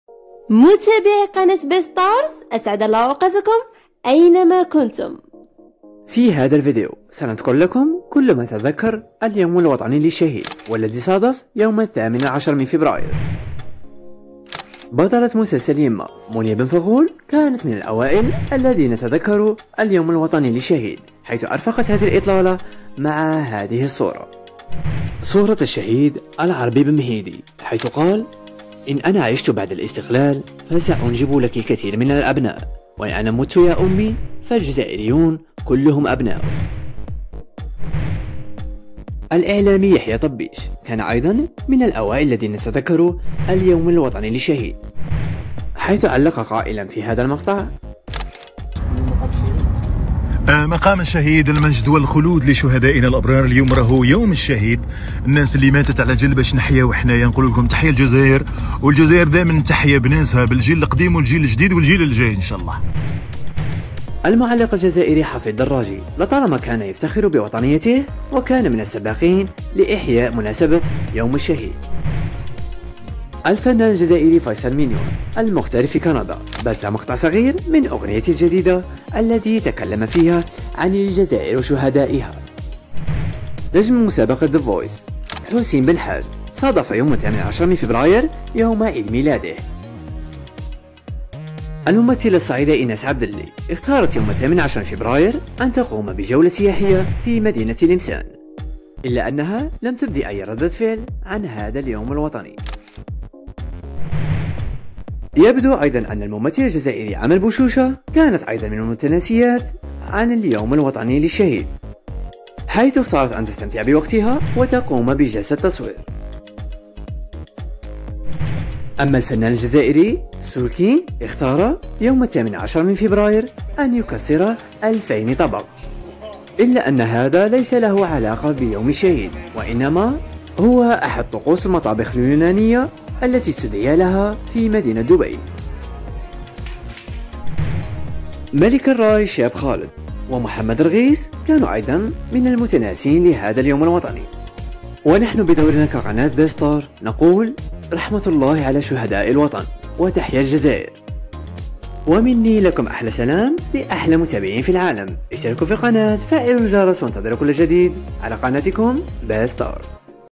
إحتفلات مشاهير الجزائر بيوم الشهيد 18فيفري،و سولكينغ يكسر2000طبق😱فيصل مينيون أغنية جديدة للشهداء 🇩🇿
إحتفلات مشاهير الجزائر بيوم الشهيد 18فيفري،و سولكينغ يكسر2000طبق!
و كان كل من مونيا بن فغول و يحيى طبيش و حفيض دراجي هم الأوغئل الذين احتفلوا بذكرى يوم الشهيد .
فيصل المينيون سيصد أغنية جديدة